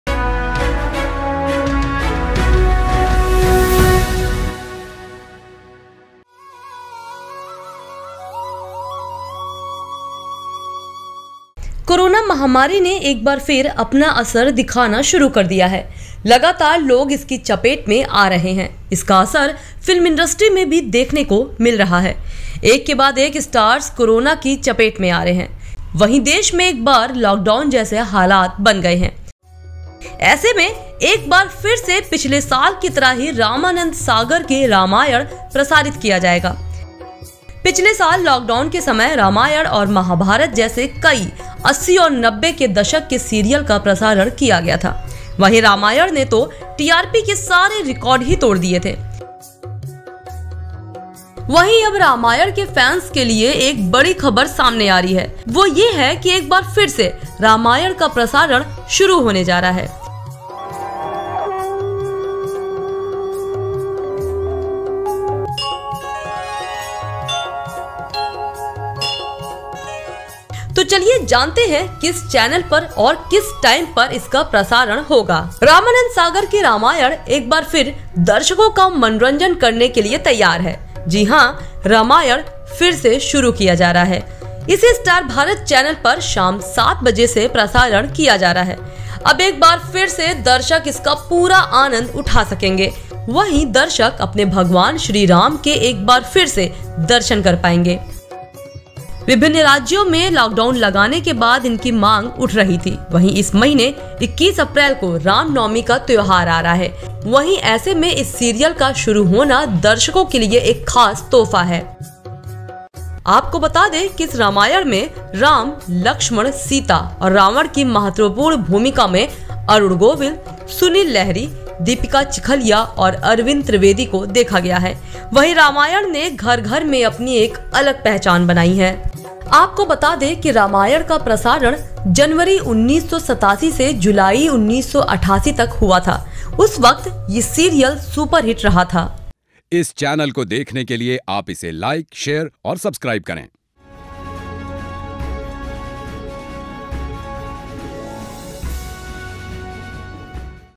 Ramyan : एक बार फिर से रामायण का प्रसारण। Ramyan Re Telecast | Ramanad Sagar
Ramyan to be Re Telecast Star Bharat Channel.  By Ramanad  Sagar
एक बार फिर से पिछले साल की तरह ही रामानंद सागर की 'रामायण' प्रसारित किया जाएगा।
पिछले साल लॉकडाउन के समय 'रामायण' और 'महाभारत' जैसे  कई 80 और 90 के दशक के सीरियल का  प्रसारण किया गया फिर से स्टार भारत चैनल में रामायण  प्रसारण होगा।